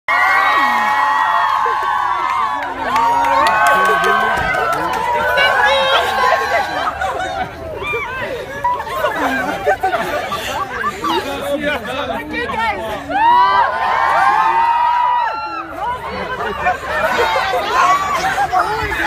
L'arrivée de Justin Trudeau à Brébeuf
C'est jeudi le 6 octobre dernier, qu'une foule d'étudiants et étudiantes du Collège Jean-de-Brébeuf attendaient avec impatience le Premier Ministre du Canada, Justin Trudeau. Il était de passage pour célébrer le 25ième anniversaire de son conventum.

Une exclusivité.